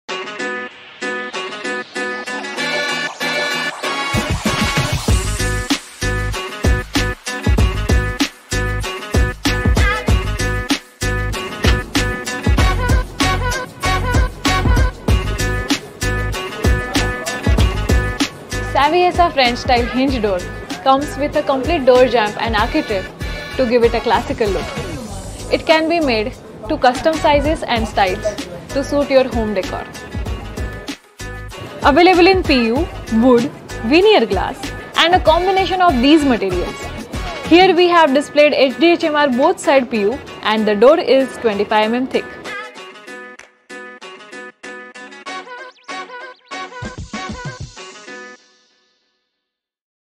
French Hinge Door
Step into a world of timeless elegance with Saviesa French Hinge Doors in PU Finish!
These doors are a symphony of style and sophistication, bringing a touch of classic French charm to your space. The flawless PU finish adds a luxurious sheen, making your room a true masterpiece. Explore the seamless beauty and practicality of these exquisite doors. Elevate your interior to new heights with Saviesa.